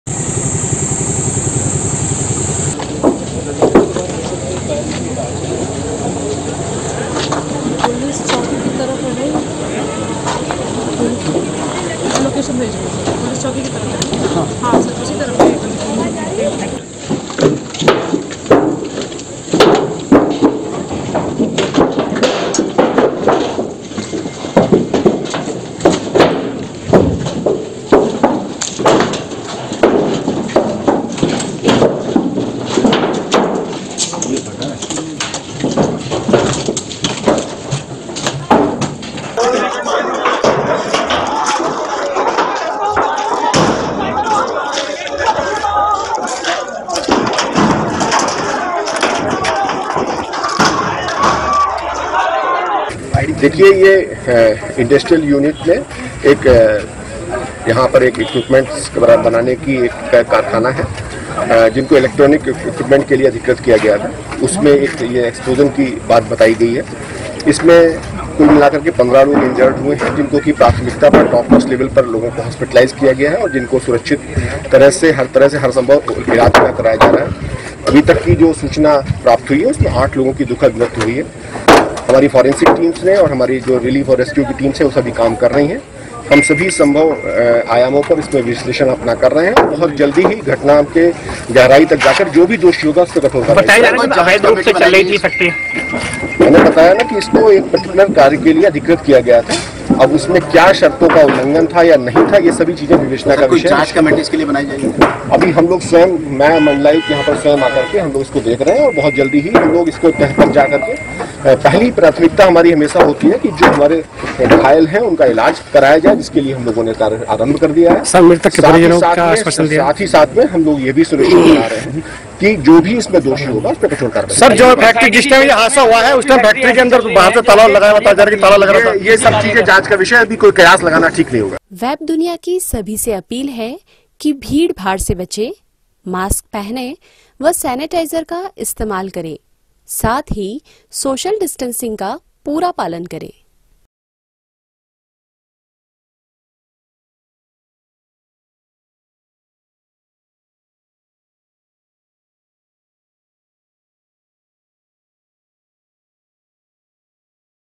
हापुड़ में बड़ा हादसा, फैक्ट्री में बॉयलर फटने से 9 मजदूरों की मौत, CM योगी ने जताया शोक
उत्तरप्रदेश के हापुड़ जिले में एक फैक्टरी का बॉयलर फटने से 8 मजदूरों की मौत हो गई जबकि कई मजदूर घायल हैं जिनका अस्पताल में उपचार चल रहा है। हापुड़ के धौलाना थाना क्षेत्र स्थित यूपीएसआईडीसी में यह बड़ा हादसा हुआ है।